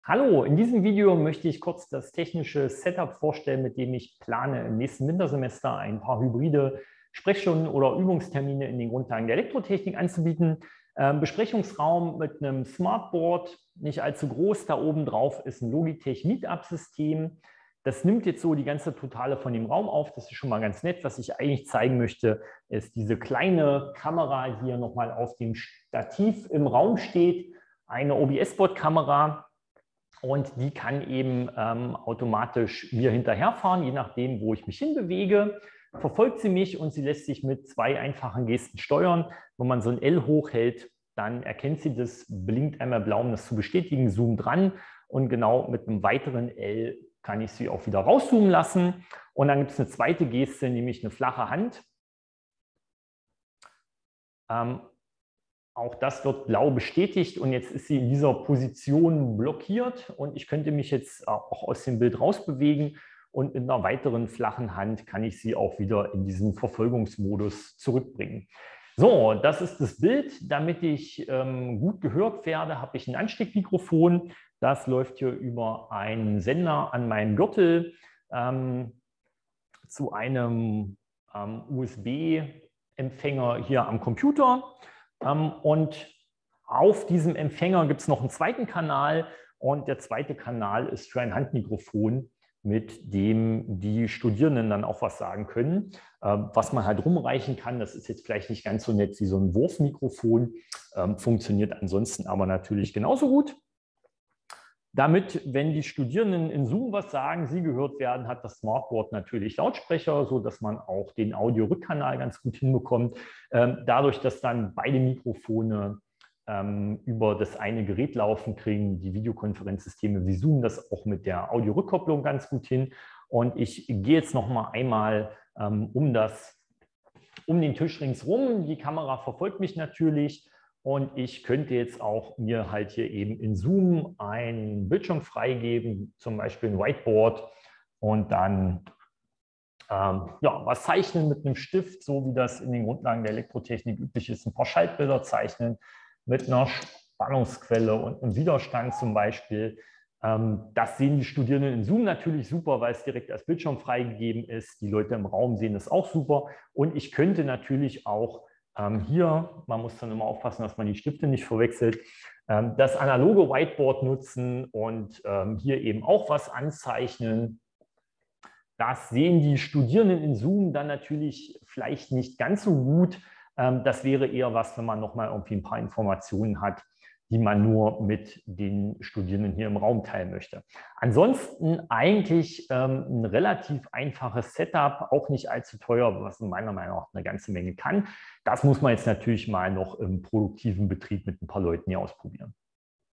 Vorstellung eines Setups für hybride Übungen/Sprechstunden/Seminare in den Ingenieurwissenschaften
Im Video stelle ich einen technischen Videokonferenzaufbau vor, mit dem sich hybride Übungen, Sprechstunden oder Seminare in ingenieurwissenschaftlichen Fächer wie der Elektrotechnik durchführen lassen. Der Seminarraum ist mit einem Smartboard mit einem Logitech-MeetUp-Videokonferenzsystem ausgestattet, dessen Kamera gut für eine Aufnahme des gesamten Raumes geeignet, aber ansonsten recht unflexibel ist. Das zugehörige Grenzflächen-Raummikrofon ist gut für Diskussionsrunden mit allen Vor-Ort-Teilnehmenden, nimmt in typischen Übungssituation mit wenigen sprechenden Personen aber auch viele unnötige Störgeräusche (Papierrascheln, Husten, etc.) auf. Zur Optimierung von Bild und Ton für die Online-Teilnehmende gibt es deshalb folgende Erweiterung. Die automatische schwenkende Kamera lässt sich durch zwei simple Gesten steuern. Ein Ansteck- und Handmikrofon sorgen für guten Ton für die Teilnehmenden im Videokonferenzsystem. Eine Audioausgabe von Online-Wortmeldungen ist über die Lautsprecher des Smartboards möglich.